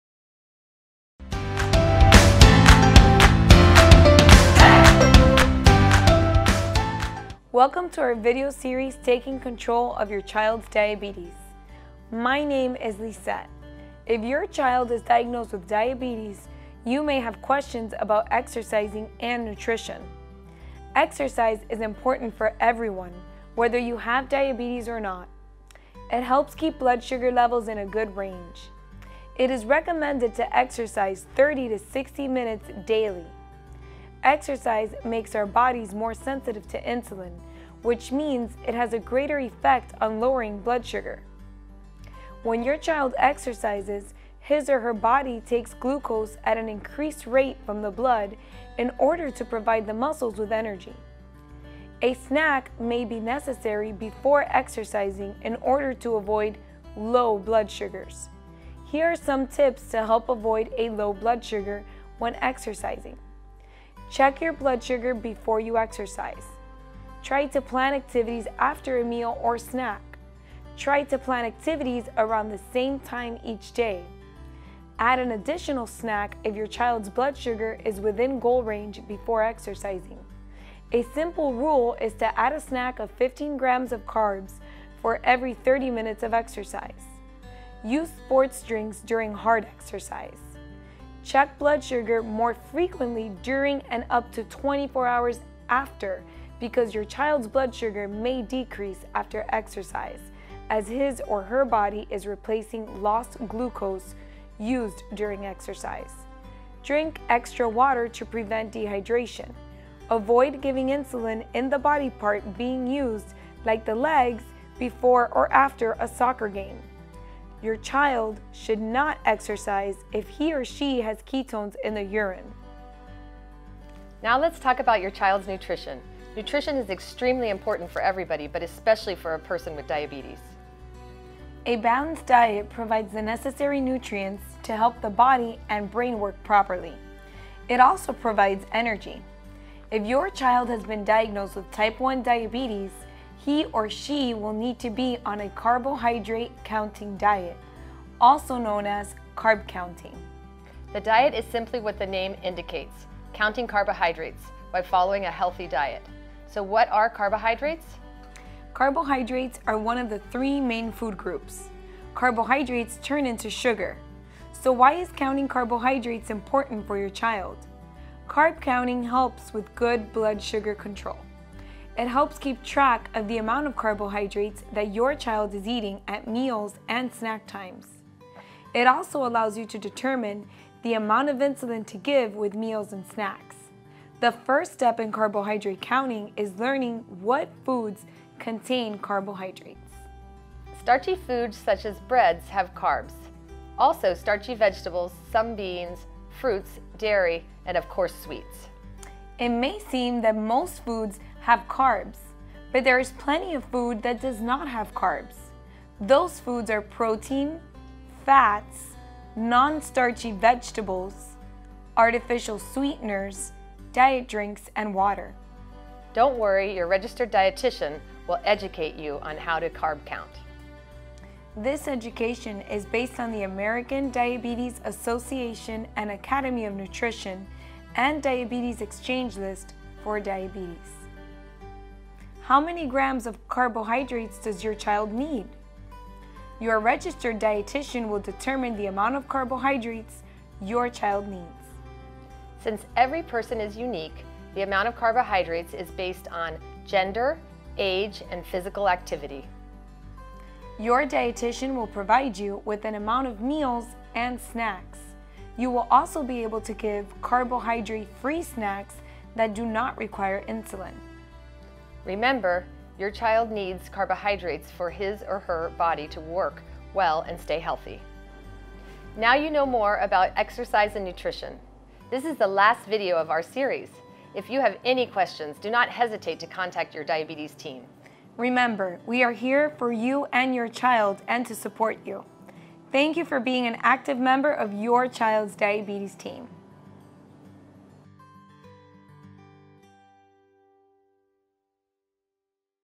Diabetes in children (9 of 9): Nutrition and Exercise
This is the last video of our diabetes series. In this video, you will learn about nutrition, exercising and carb counting. The video answers the following questions:  What are carbohydrates? How many grams of carbohydrates does your child need? What foods have carbohydrates? How do you count carbohydrates? This video also provides recommendations and ideas on “free food” snacks for your child.